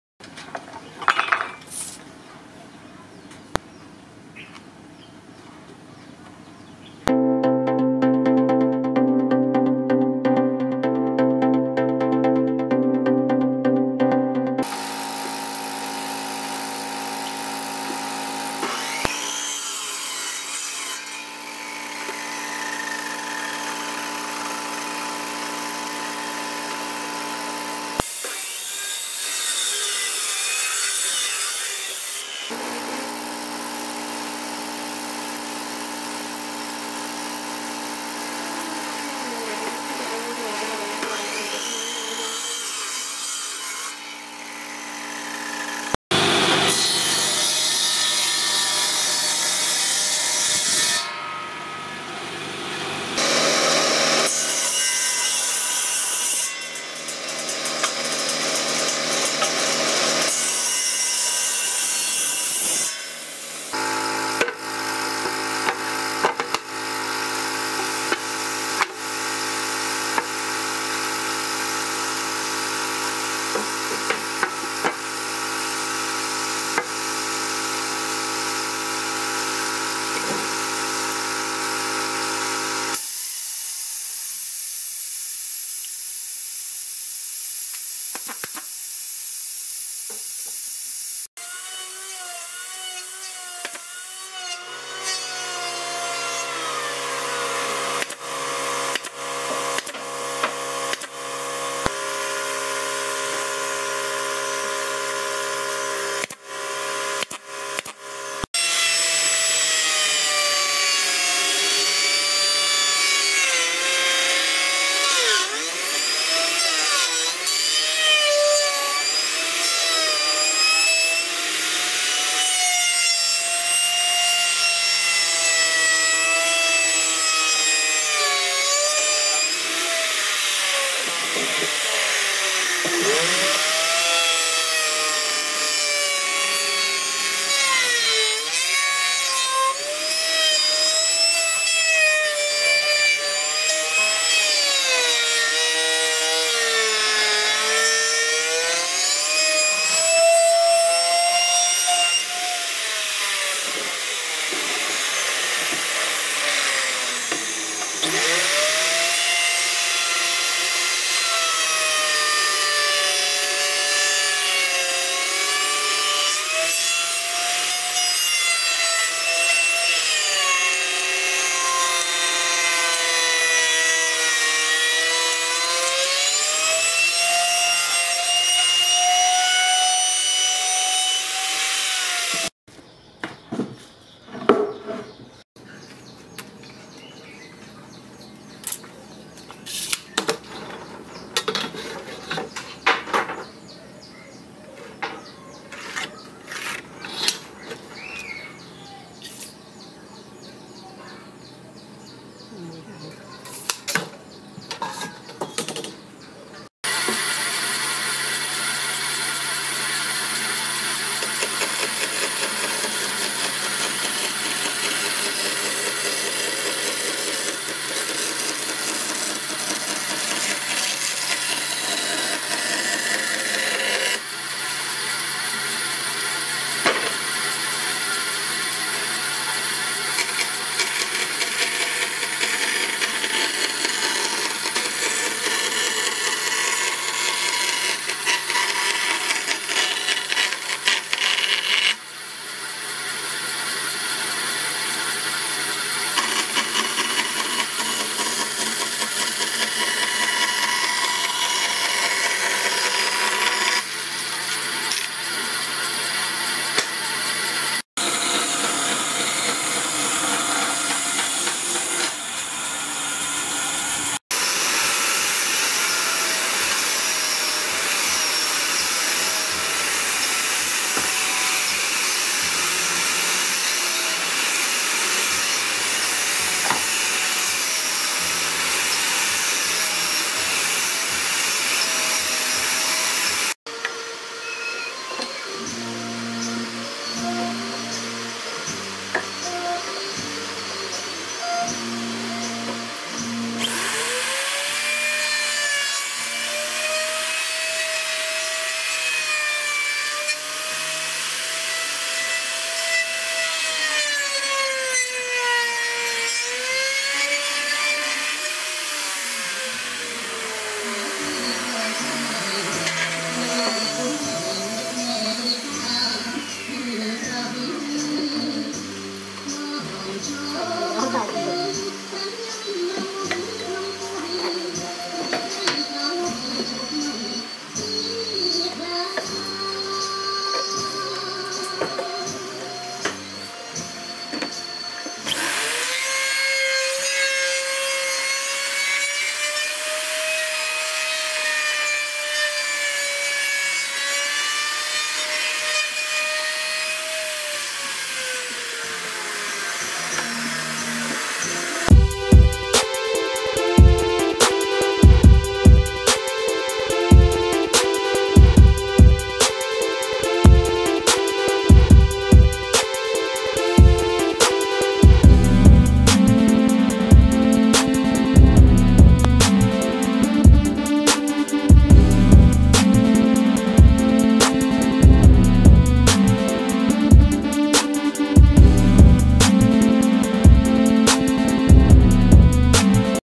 Wooden boxes are made by skilled carpenters
In this video I will show you how to make a wooden box clean and strong.
Please help me edit.